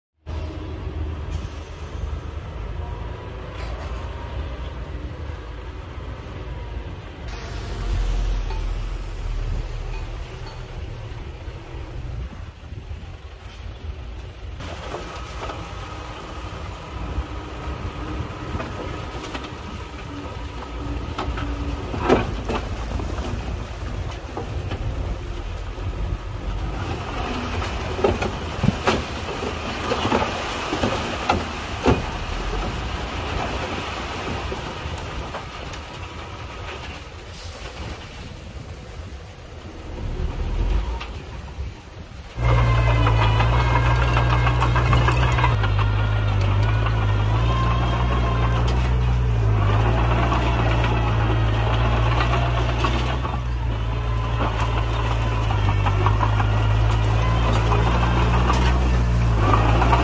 Amazing Dump Truck 5T Pour Stone & land.
Are you looking for an efficient way to move dirt? In this video, You'll see a bulldozer in action, pushing dirt with ease and speed. Watch as it effortlessly moves large piles of earth while still maintaining control and accuracy. See why bulldozer are the go-to chose for construction project among  the world.